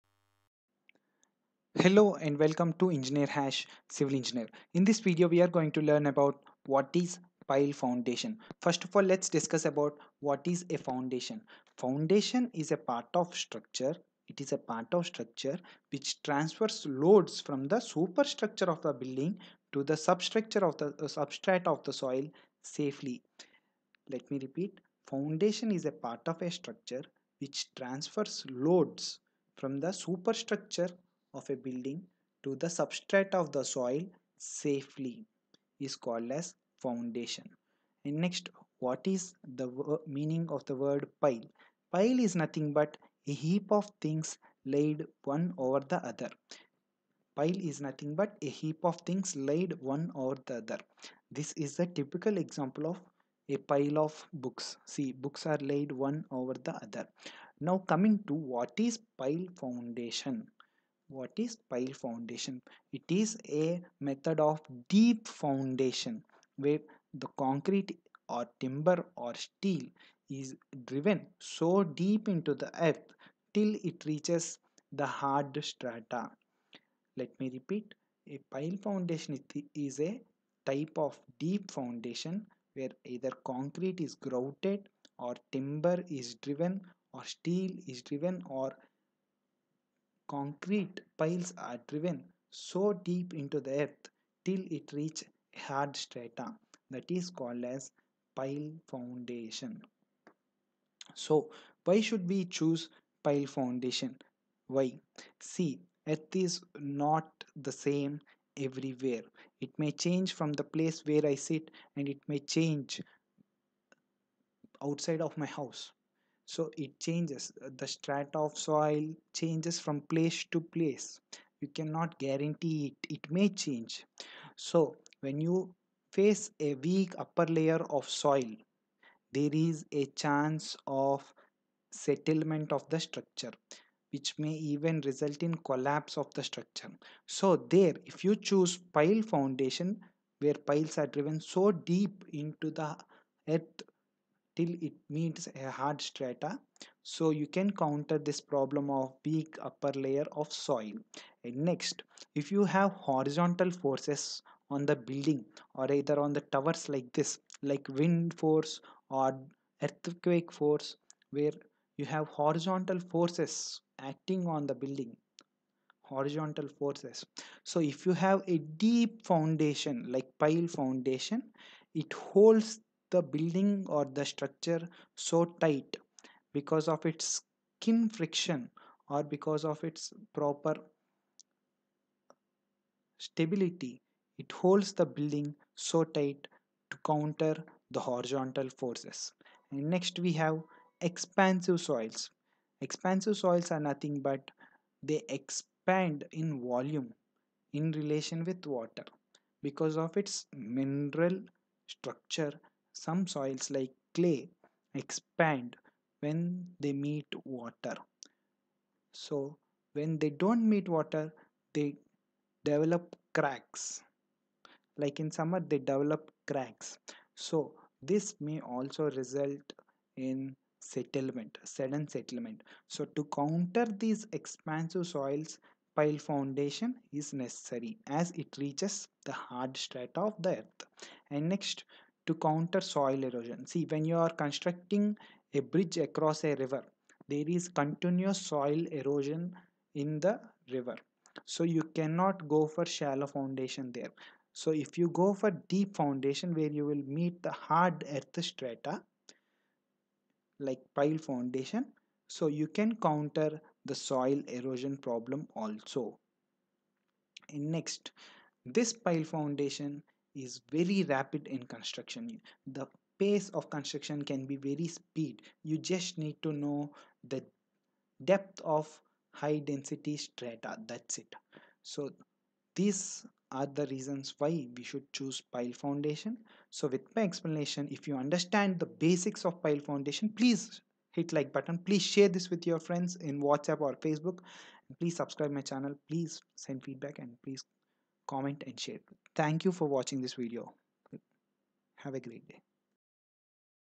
What is Pile Foundation??
In this video I have discussed the meaning of pile foundation, the reasons to choose pile foundation.